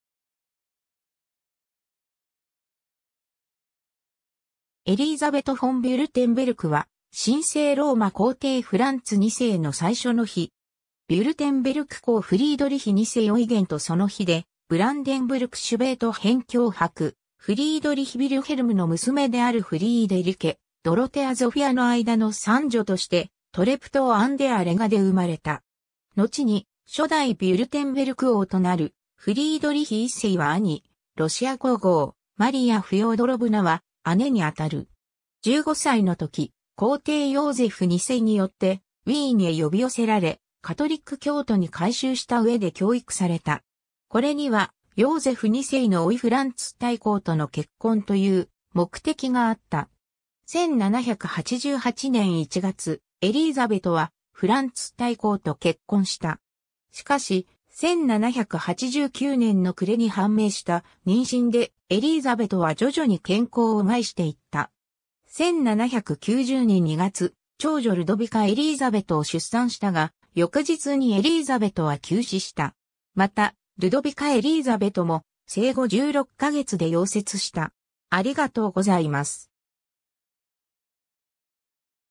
エリーザベト・フォン・ヴュルテンベルク
ヴュルテンベルク＝ヴィンネンタール家
エリーザベト・フォン・ヴュルテンベルク （Elisabeth Wilhelmine Louise von Württemberg, 1767年4月21日 - 1790年2月18日）は、神聖ローマ皇帝フランツ2世の最初の妃。
 ヴュルテンベルク公フリードリヒ2世オイゲンとその妃でブランデンブルク＝シュヴェート辺境伯フリードリヒ・ヴィルヘルムの娘であるフリーデリケ・ドロテア・ゾフィアの間の三女として、トレプトウ・アン・デア・レガ（現ポーランド・トシェビャトゥフ）で生まれた。
のちに初代ヴュルテンベルク王となるフリードリヒ1世は兄、ロシア皇后マリア・フョードロヴナは姉にあたる。
 15歳の時、皇帝ヨーゼフ2世によってウィーンへ呼び寄せられ、カトリック教徒に改宗した上で教育された。
これには、ヨーゼフ2世の甥フランツ大公との結婚という目的があった。
 1788年1月、エリーザベトはフランツ大公と結婚した。
しかし、1789年の暮れに判明した妊娠で、エリーザベトは徐々に健康を害していった。
1790年2月、長女ルドヴィカ・エリーザベトを出産したが、翌日にエリーザベトは急死した。
また、ルドヴィカ・エリーザベトも生後16ヶ月で夭折した。